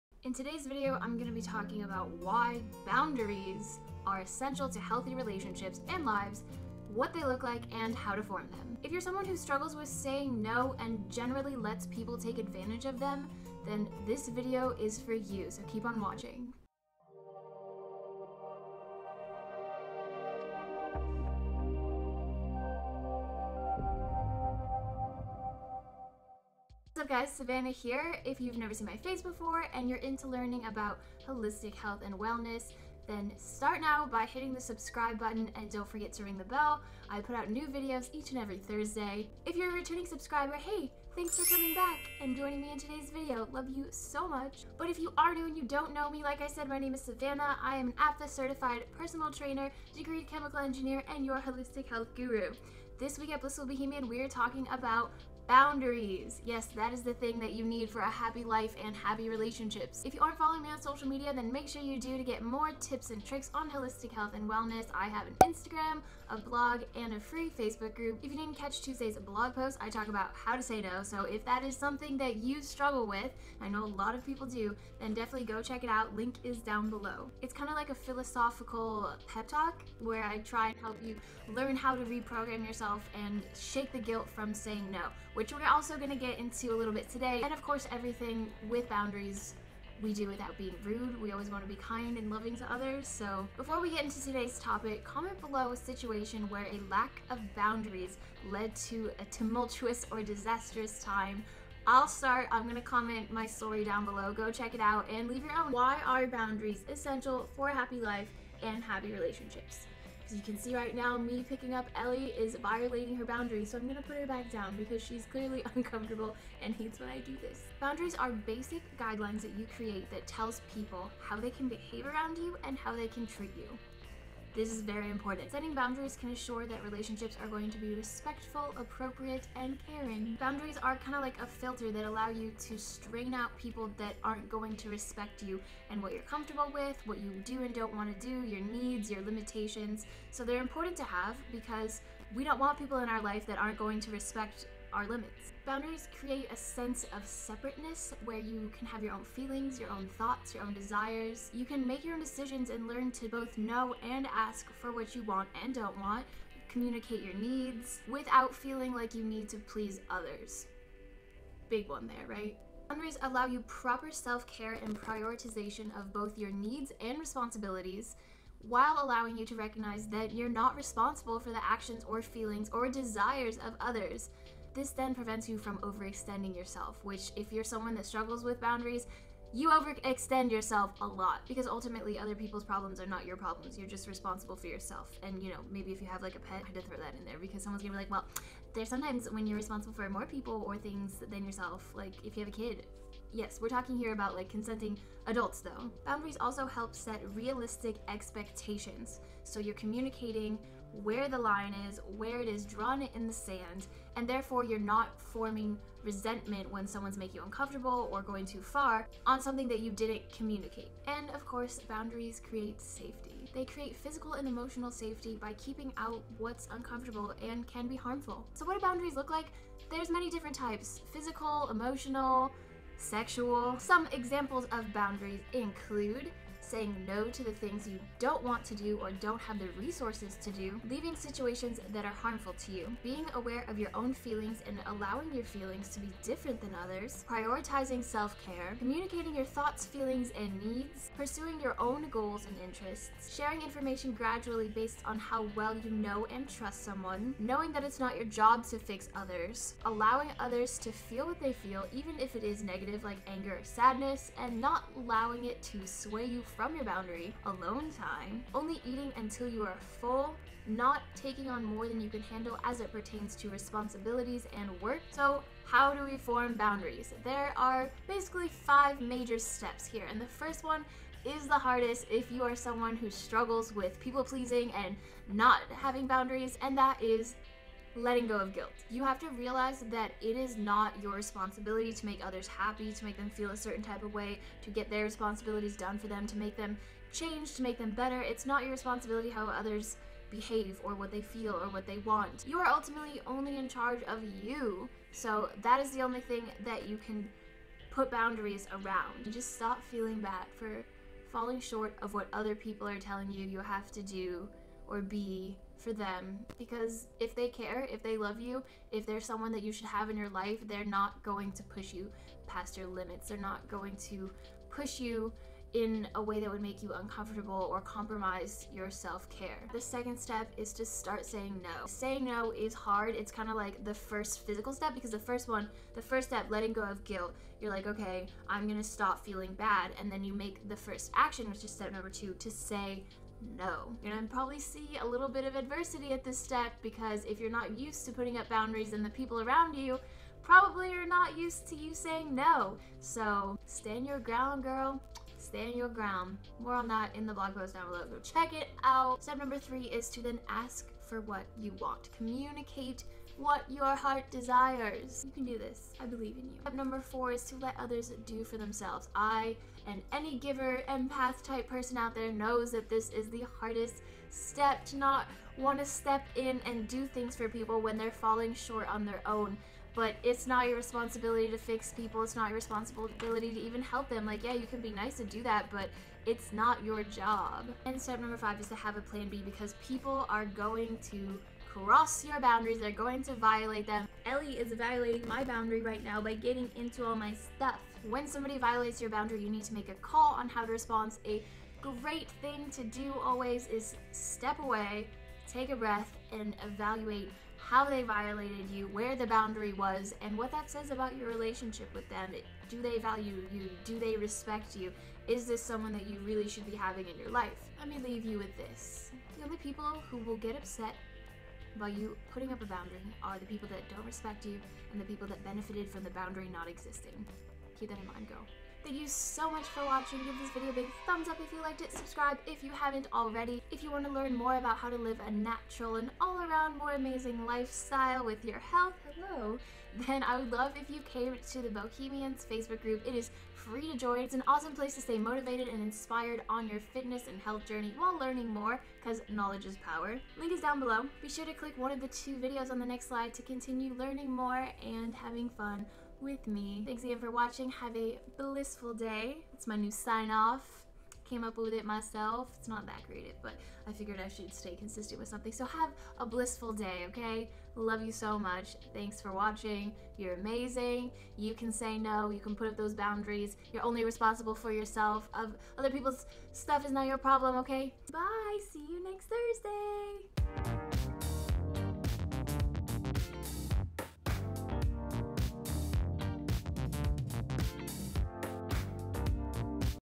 Why THIS is Essential to Healthy Lives & Relationships
Knowing how to set boundaries is essential an essential skill in having a healthy lifestyle and in forming healthy relationships. You have got to take care of yourself first, before anyone else. Can’t pour from an empty cup.